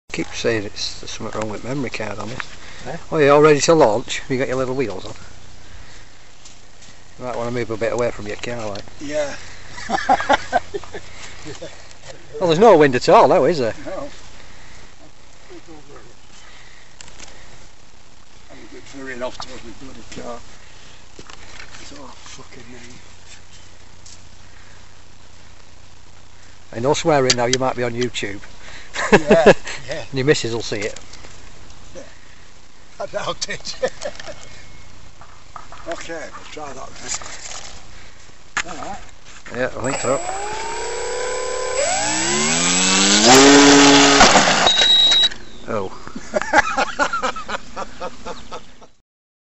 ACRO MASTER TAKE OFF USING HOME MADE BOGEY...DIDNT WORK TOO WELL
Here is a very short video of my mate Stan on middleton sands testing out is Bogey with his Acro Master. not too good really lol